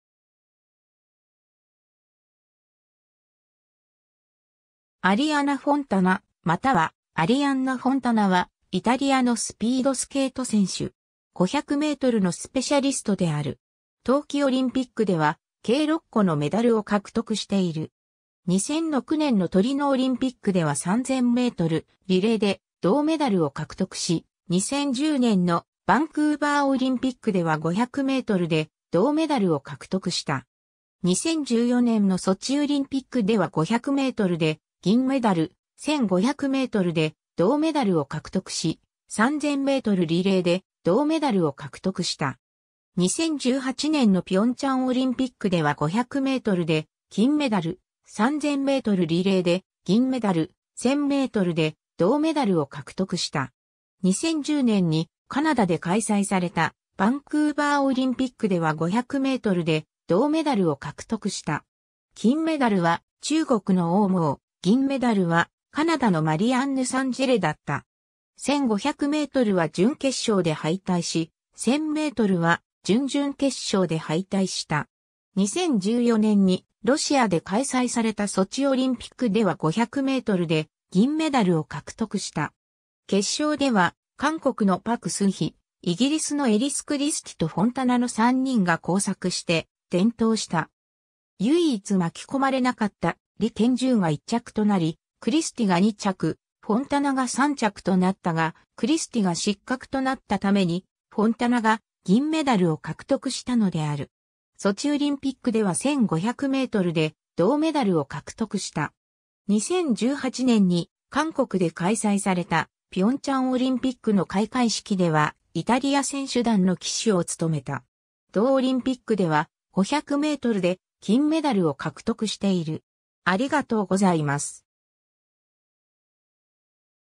アリアナ・フォンタナ
アリアナ・フォンタナまたはアリアンナ・フォンタナ（Arianna Fontana、女性、1990年4月14日 - ）は、イタリアのスピードスケート選手（ショートトラック）。
500mのスペシャリストである。
 冬季オリンピックでは計6個のメダルを獲得している。
2006年のトリノオリンピックでは3000メートルリレーで銅メダルを獲得し、2010年のバンクーバーオリンピックでは500mで銅メダルを獲得した。
2014年のソチオリンピックでは500mで銀メダル、1500mで銅メダルを獲得し、3000mリレーで銅メダルを獲得した。
2018年の平昌オリンピックでは500mで金メダル、3000mリレーで銀メダル、1000mで銅メダルを獲得した。
 2010年にカナダで開催されたバンクーバーオリンピックでは500mで銅メダルを獲得した。
金メダルは中国の王濛、銀メダルはカナダのマリアンヌ・サン＝ジェレだった。
1500mは準決勝で敗退し、1000mは準々決勝で敗退した。
 2014年にロシアで開催されたソチオリンピックでは500mで銀メダルを獲得した。
決勝では韓国のパク・スンヒ、イギリスのエリス・クリスティとフォンタナの3人が交錯して転倒した。
唯一巻き込まれなかった李堅柔が1着となり、クリスティが2着、フォンタナが3着となったが、クリスティが失格となったためにフォンタナが銀メダルを獲得したのである。
ソチオリンピックでは1500mで銅メダルを獲得した。
 2018年に韓国で開催された平昌オリンピックの開会式ではイタリア選手団の旗手を務めた。
同オリンピックでは、500mで金メダルを獲得している。